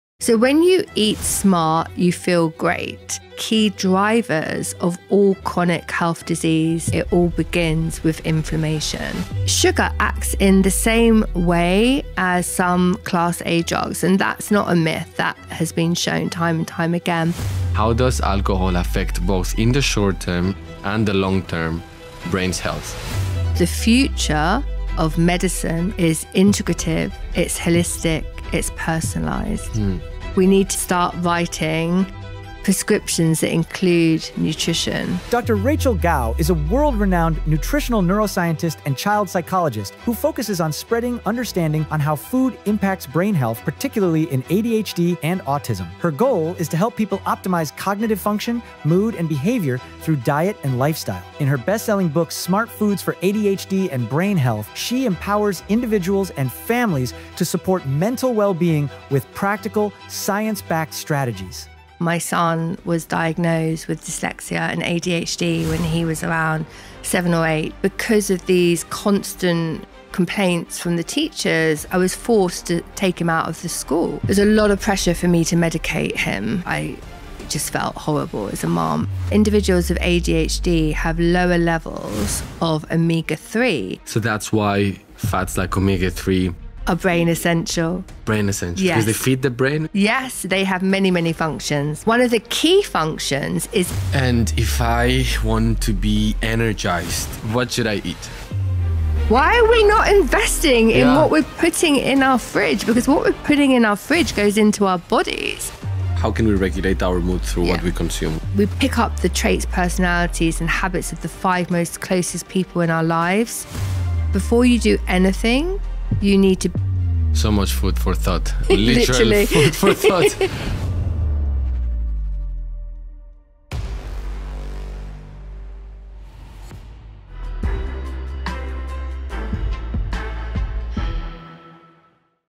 Most people think food affects their body
Most people think food affects their body. Very few realise it is shaping their brain. In this episode of Thriving Minds, I spoke with Dr Rachel Gow, one of the world’s leading nutritional neuroscientists. We explored how what you eat influences focus, mood, anxiety, motivation, and even long term mental health. This conversation completely reframed how I think about food, not as fuel for appearance, but as architecture for the brain. This one will change how you look at your plate. . . . .